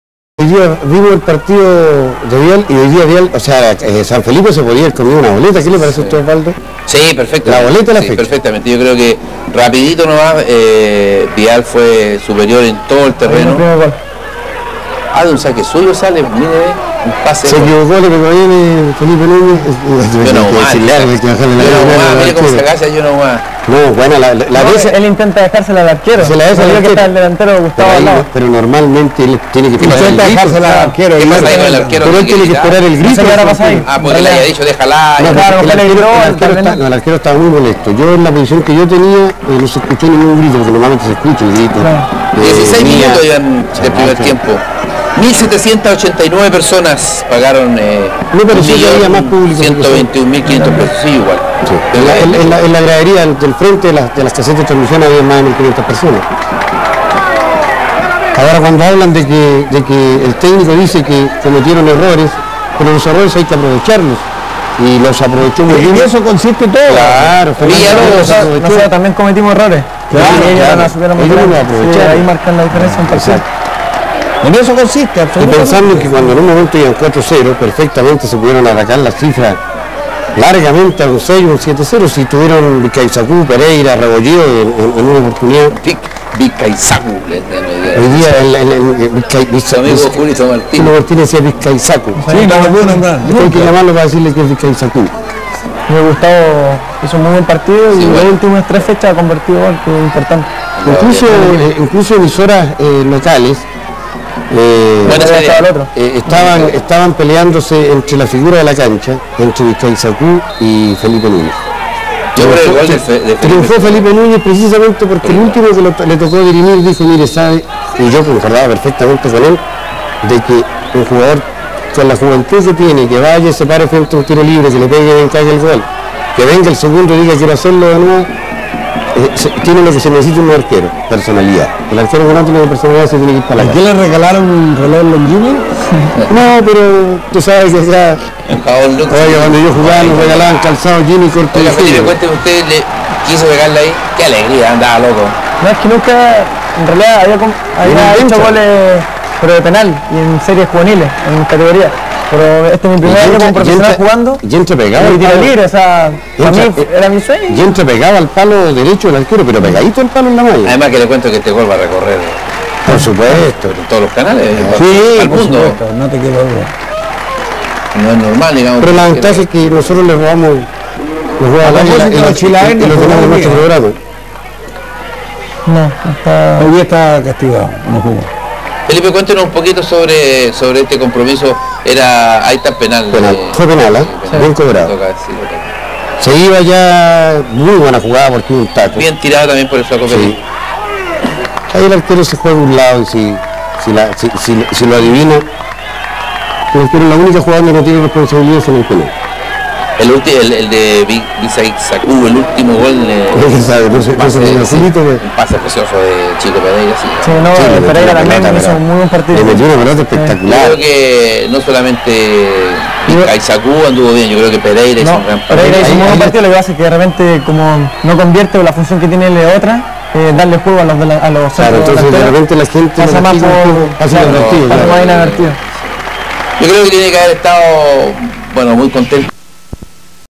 Arturo Fernández Vial VS Unión San Felipe 2000
Resumen
Arturo Fernández Vial 4-1 Unión San Felipe
Goles
Arturo Fernández Vial: Gustavo Biscayzacú (2), Manuel Villalobos, Felipe Núñez
Unión San Felipe: Pérez
Fecha 18 Campeonato De Primera B 2000
2°Fase